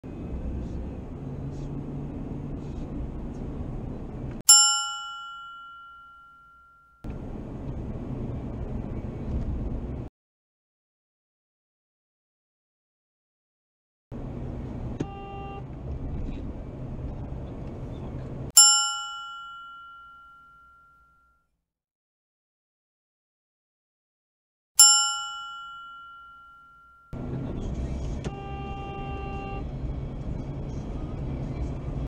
Two idiots in a matter of seconds
5/9/19 Worcester MA. I don't know what it is with this stretch of Velmost street but people are ALWAYS just whipping out into traffic. Today I got two in a matter of seconds. The first guy at least pulled out into the middle lane (guy to my left didn't like that though) but the second one - an older lady in a red Mercedes - doesn't even bother to stop at the stop sign and just shoots out into my lane like a complete moron.